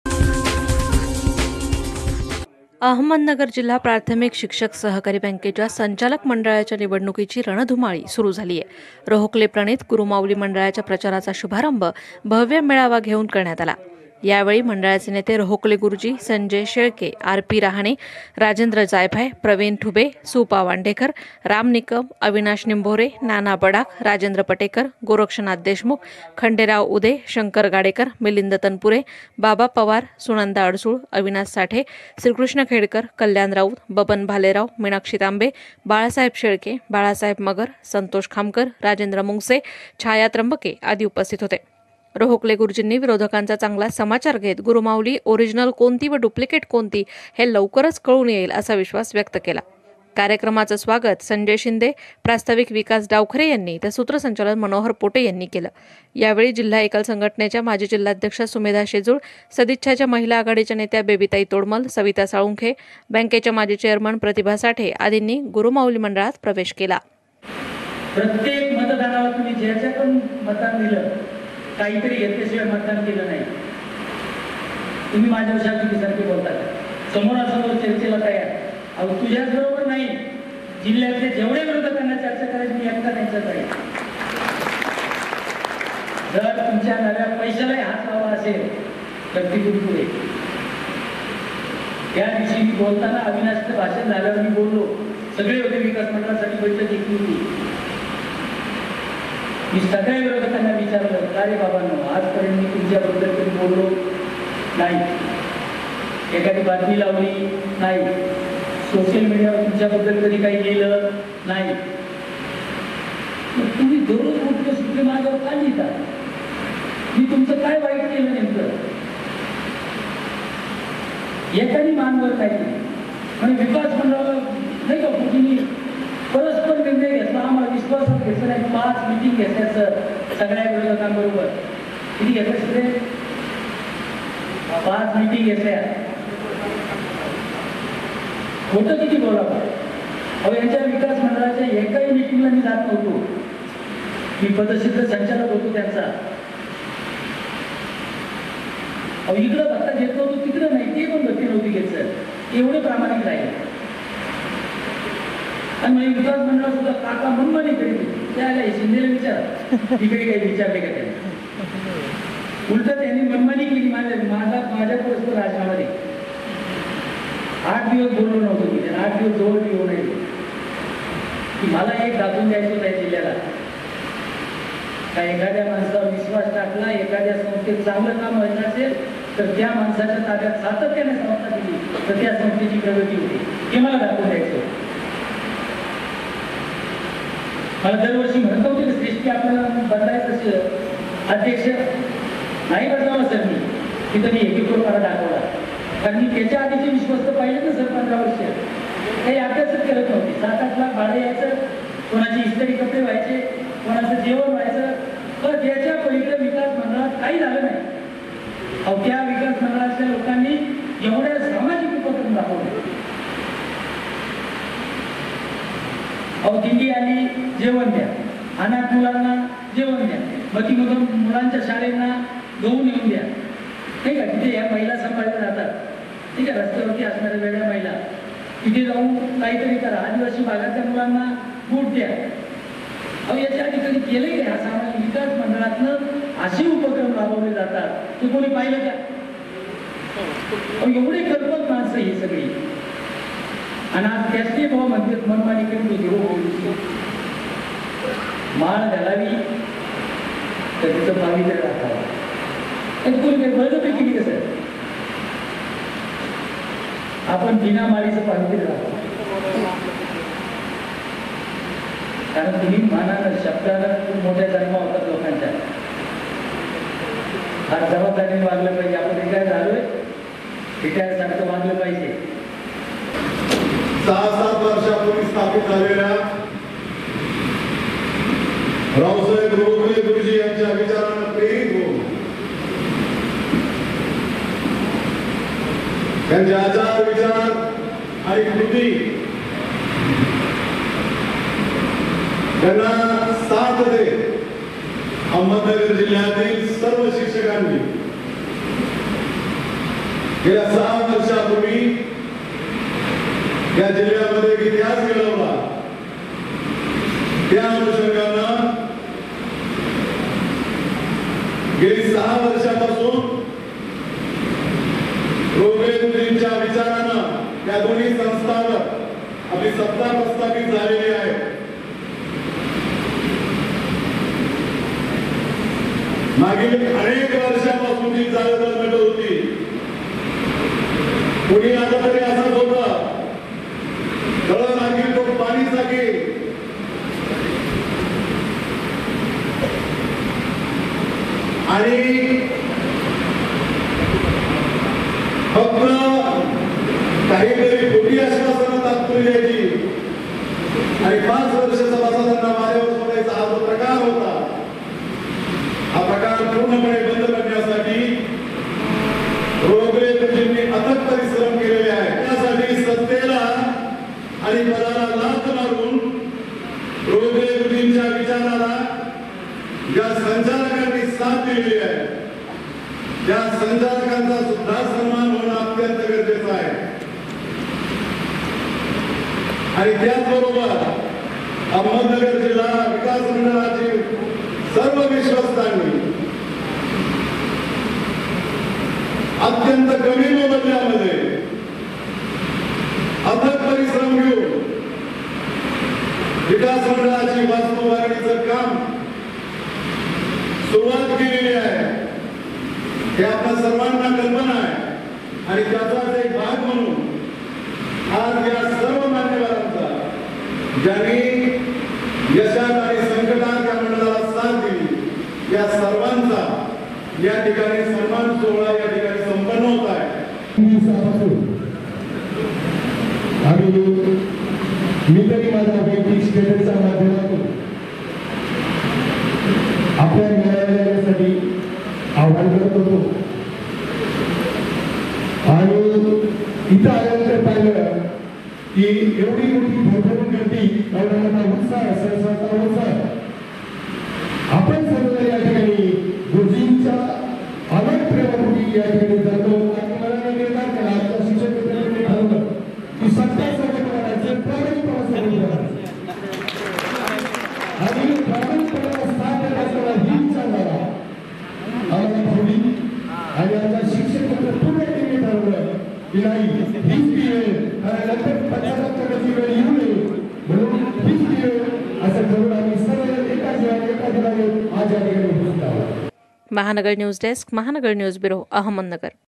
शिक्षक बँक निवडणुकीसाठी रोहोकले यांच्या गुरुमाउली मंडळाच्या प्रचाराचा शुभारंभ
रोहोकले गुरुजींचे विरोधकांच्या टिकेला जोरदार प्रत्युत्तर